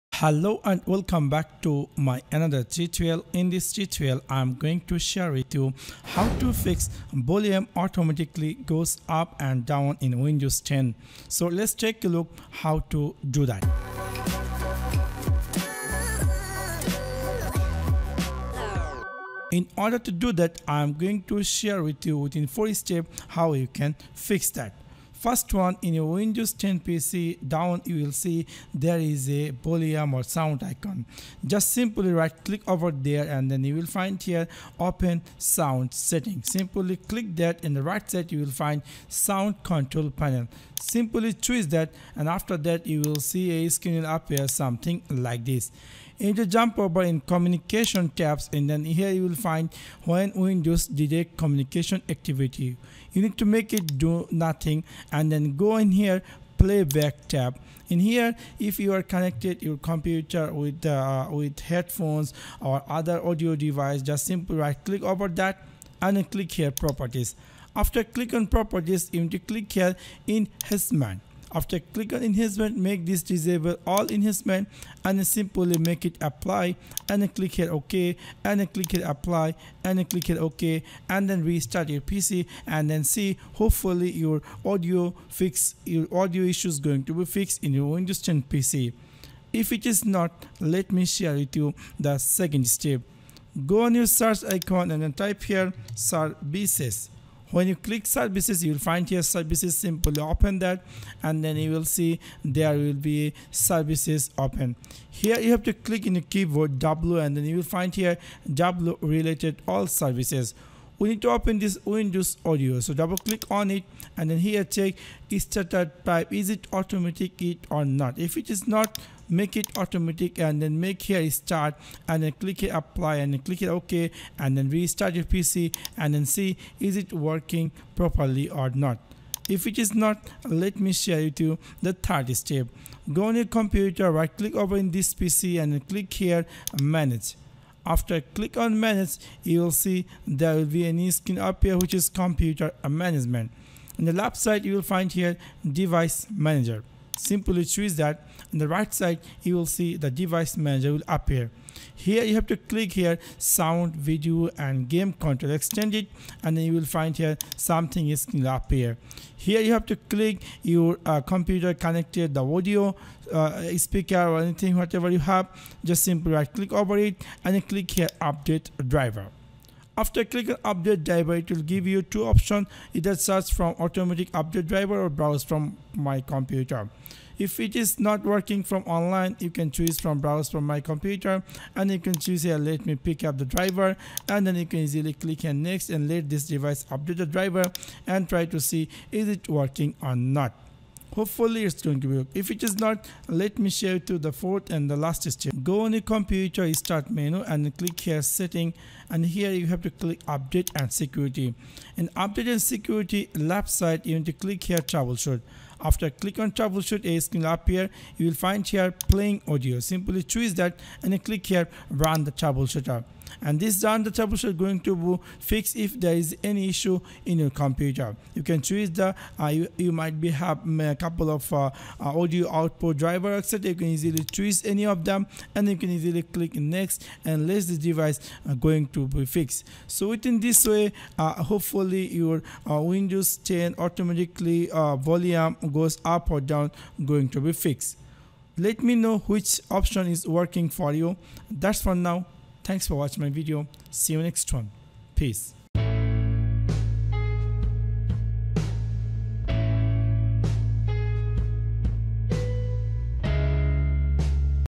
How to Fix Volume Automatically Goes Up & Down in Windows 10 | Volume Too Low
FIX Volume Too Low or high on Windows.

Low or high PC volume isn’t an entirely uncommon scenario in Windows. In most cases, there are relatively straightforward fixes for software sound that doesn’t match up with the Windows volume bar level. If your laptop’s or desktop’s audio is lower or higher than what you would usually expect, these are a few resolutions that will probably restore the volume level in Windows.